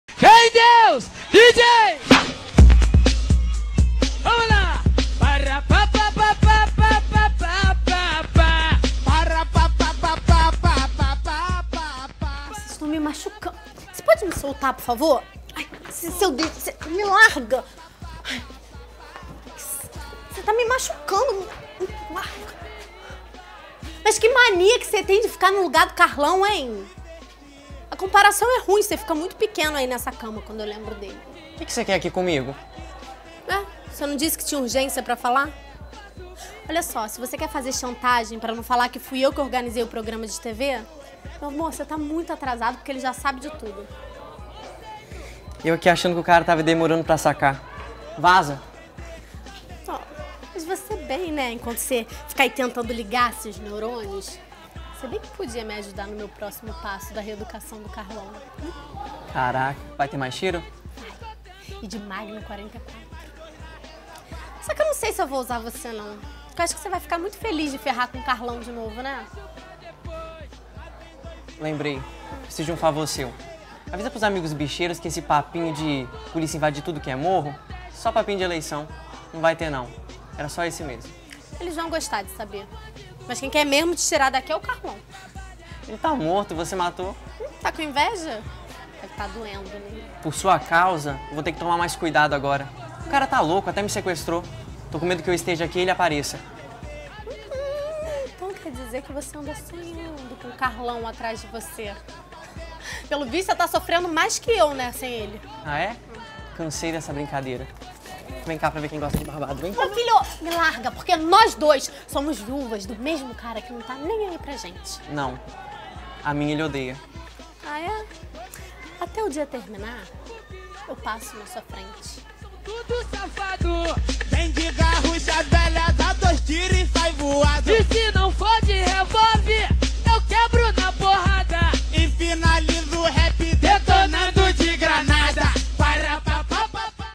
Pedro Balista e Marcela Barrozo - Cena da novela Pecado Mortal
Cena gravada no Workshop de Interpretação para TV com Marcos Reis e gravação de cena com Marcela Barrozo.

Gravação realizada no dia 18/10/2014.